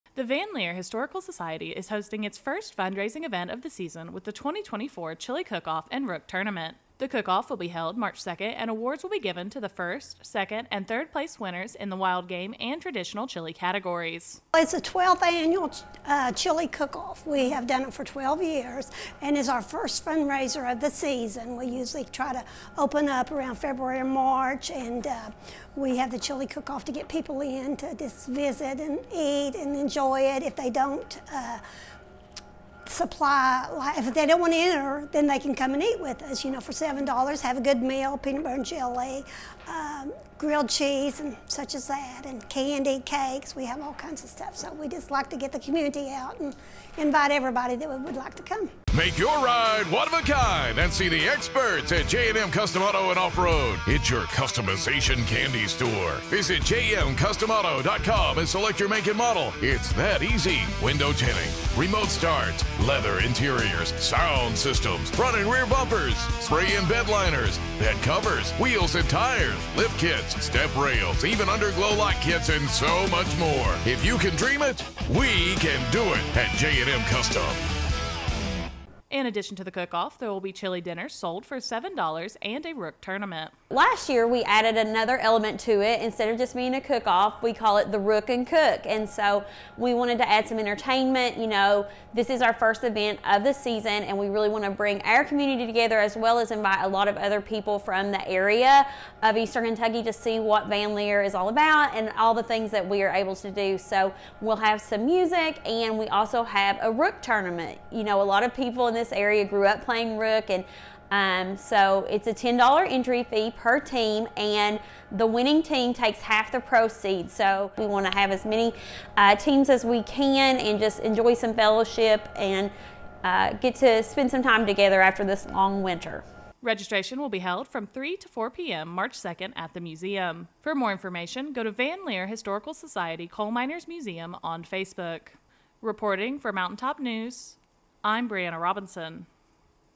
Van Lear 2024 Chili Cook Off & Rook Tournament is set for March 2
The Van Lear Historical Society will host its first fundraiser of the season with the 2024 Chili Cook Off & Rook Tournament on March 2.